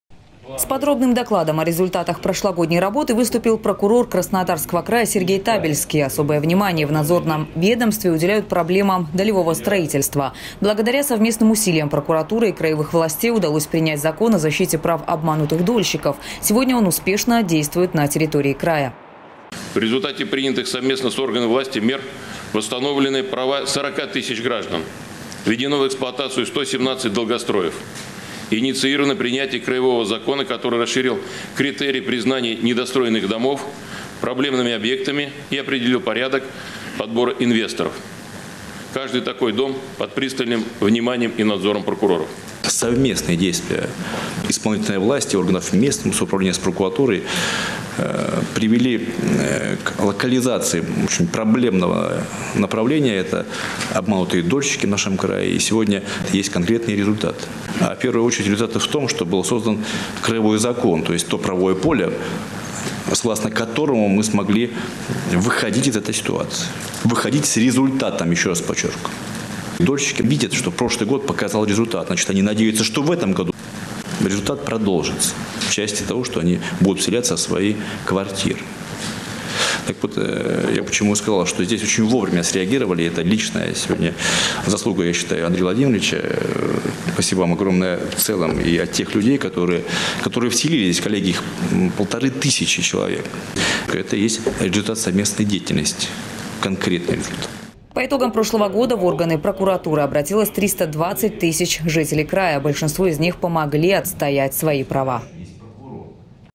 Краевая прокуратура подвела итоги работы за прошлый год. Новости Эфкате
В расширенном заседании коллегии принял участие заместитель генерального прокурора России Андрей Кикоть и глава Кубани Вениамин Кондратьев.
С подробным докладом о результатах прошлогодней работы выступил прокурор Краснодарского края Сергей Табельский. Особое внимание в надзорном ведомстве уделяют проблемам долевого строительства. Благодаря совместным усилиям прокуратуры и краевых властей, удалось принять закон о защите прав обманутых дольщиков. Сегодня он успешно действует на территории края.